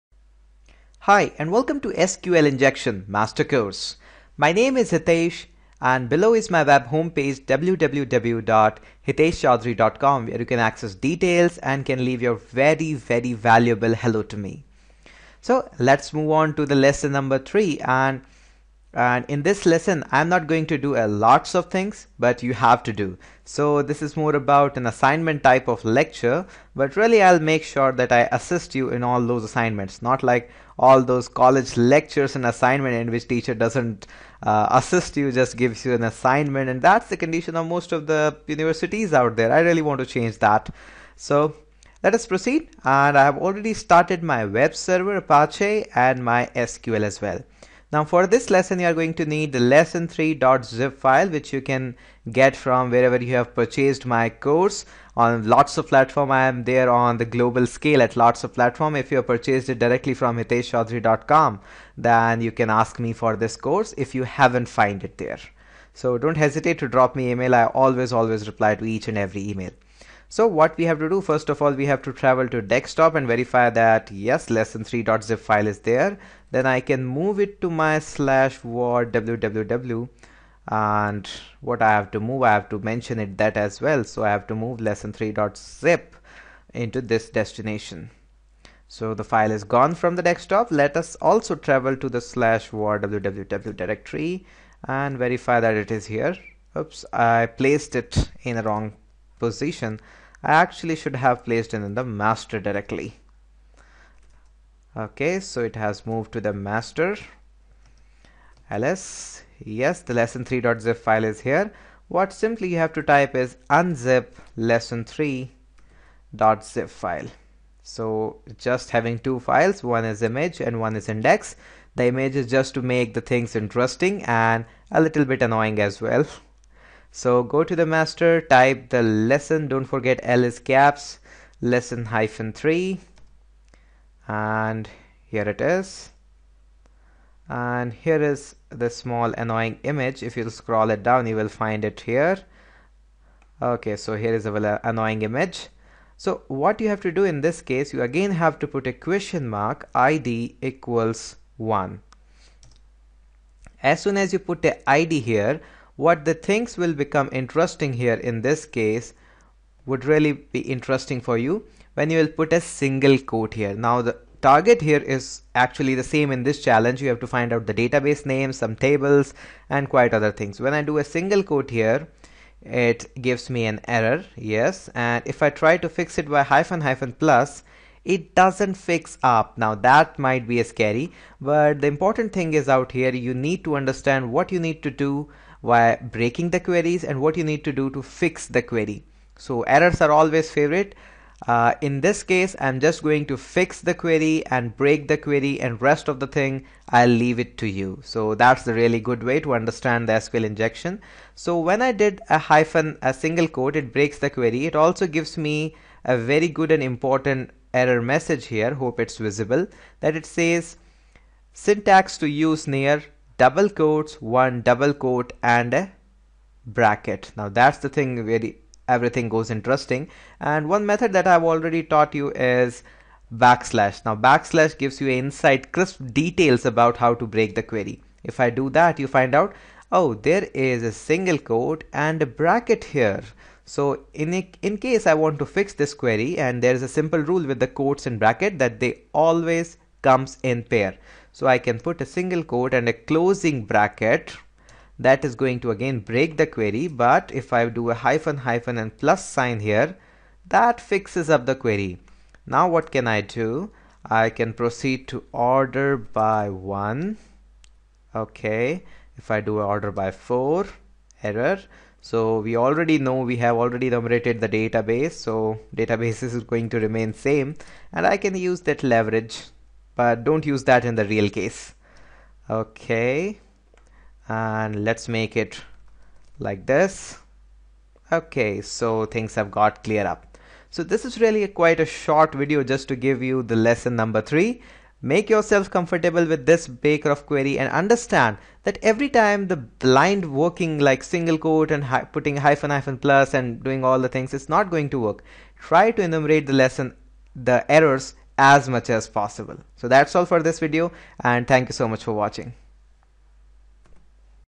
Lecture - 14 | SQL Injection Challenge-Solution 3 | CurioCraft SQL Series
Welcome to Lecture 14 of our comprehensive SQL Injection series on CurioCraft! In this episode, we present you with an exciting challenge that tests your SQL Injection skills. 🕵️‍♂️💻

🔥 Challenge: Dive into a realistic scenario where a web application is vulnerable to SQL Injection. Your mission is to exploit the vulnerability using your knowledge of SQL Injection techniques. Follow along as we guide you through the process step by step.

🛡️ Solution: Once you've given the challenge your best shot, we'll reveal the solution, explaining each step and the thought process behind it. This hands-on approach will enhance your understanding of how SQL Injection vulnerabilities work and how to prevent them.

🔍 What You'll Learn:

    Practical application of SQL Injection techniques
    Analyzing and exploiting vulnerabilities in web applications
    Strategies for securing databases against malicious attacks

Sharpen your cybersecurity skills and become a savvy defender of data integrity. Whether you're a budding ethical hacker, a web developer, or someone who's eager to explore the world of database security, this lecture is designed to challenge and educate.